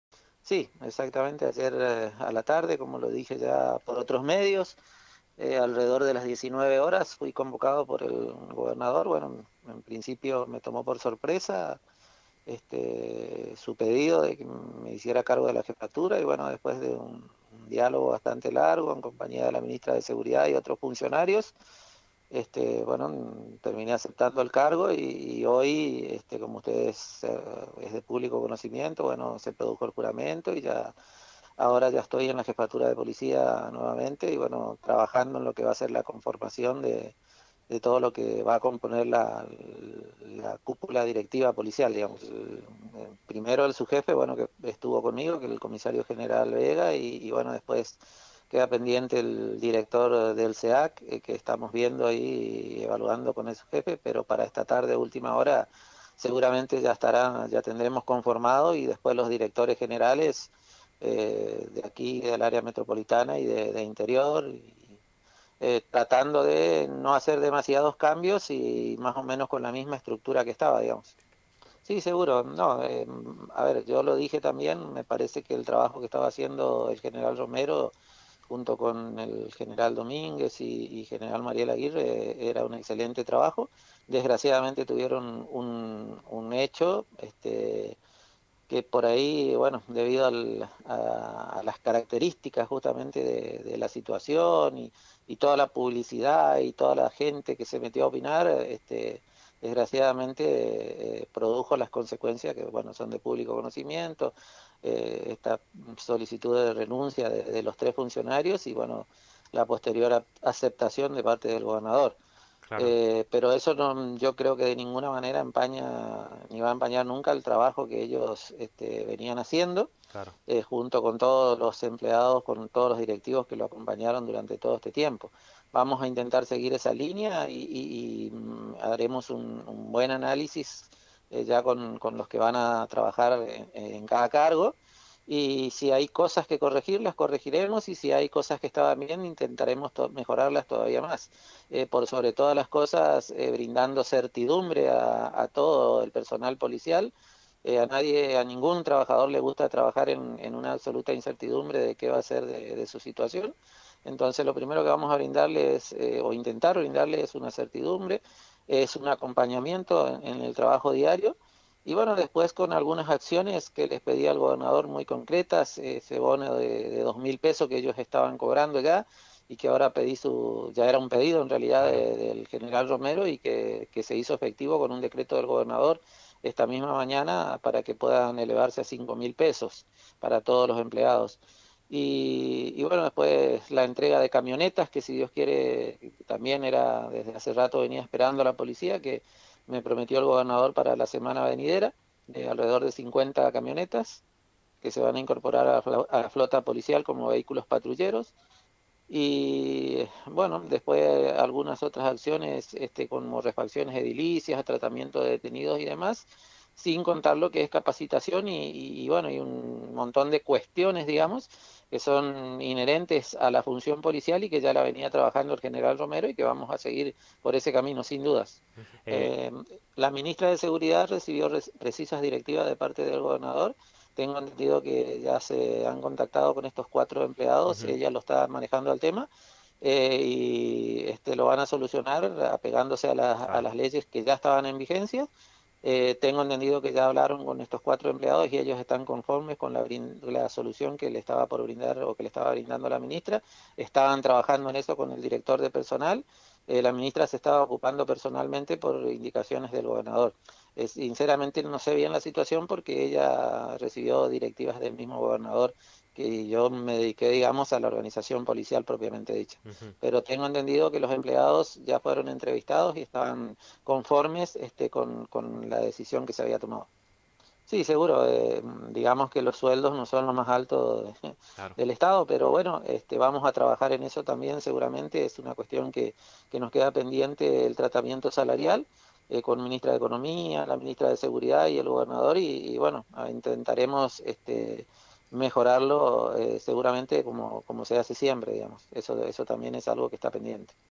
Nuevas autoridades en la cupula policial del Chaco
El nuevo Jefe de la Policía, Ariel Acuña, explicó que se sorprendió ante el pedido del Gobernador, además adelantó “si hay cosas que corregir, corregiremos” y que “el Gobernador prometió 50 camionetas” (serán entregadas la semana que viene). En referencia a los Policías acusados explicó que tiene entendido que la Ministra de Seguridad (Gloria Zalazar) solucionó el problema y habrían quedado conformes.

-Ariel Acuña, Jefe de la Policia del Chaco.